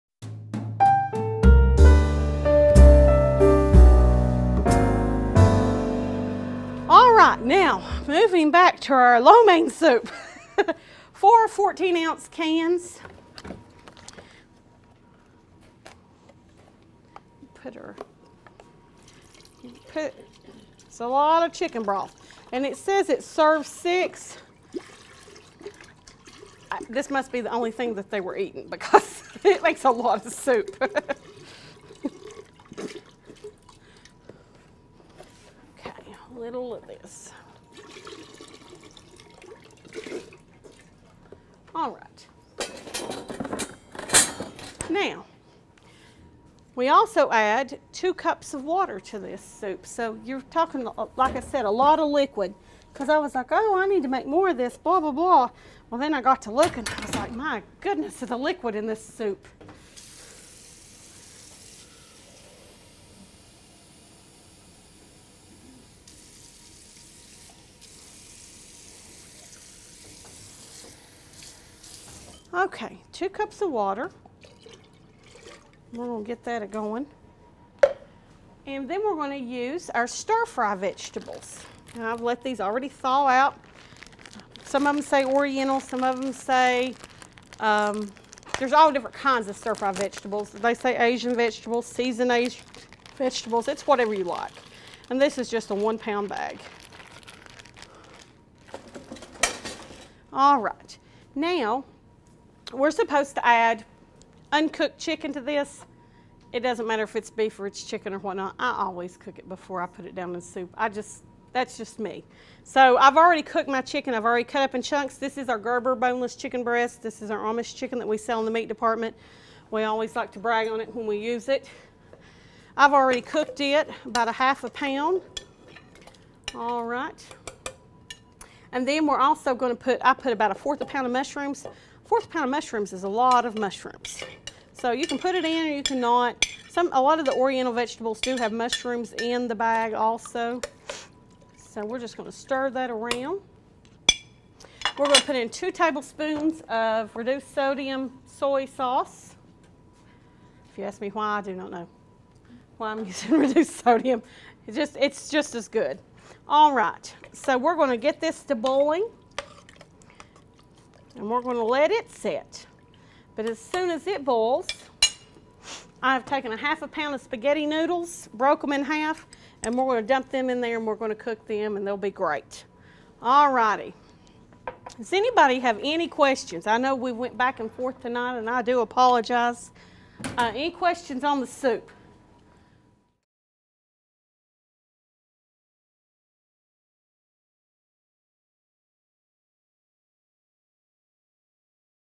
Recipe: Lo Mein Soup
4-(14oz) cans ready to serve chicken broth
2 cups water
1(16oz) package frozen stir fry vegetables, thawed
½ lb boneless, skinless, chicken (cut into ½ " chunks)
¼ lb mushrooms, thinly sliced
2T light soy sauce
½ lb spaghetti (broke in half)

In a soup pot, combine all the ingredients, except the spaghetti. Bring to a boil over a
medium-high heat. Add the spaghetti and boil 8-10 minutes, or until the spaghetti is cooked
and no pink remains on the chicken.